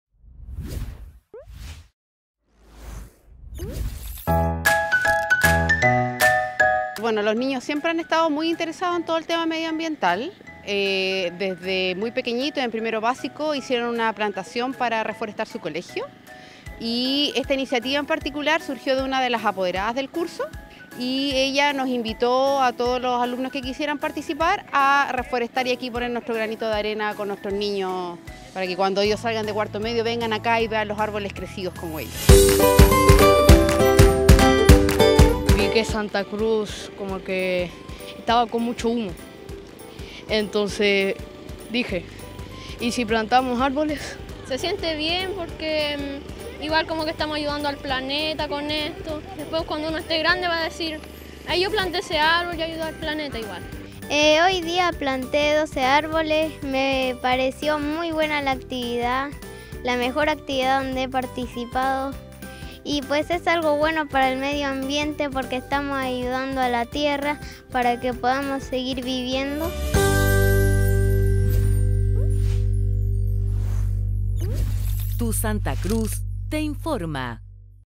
ALUMNOS DEL COLEGIO SANTA CRUZ UNCO, CONTRIBUYEN AL CUIDADO DEL MEDIO AMBIENTE
Estudiantes del Colegio Santa Cruz de Unco, realizaron plantación de árboles en Parque Colchagua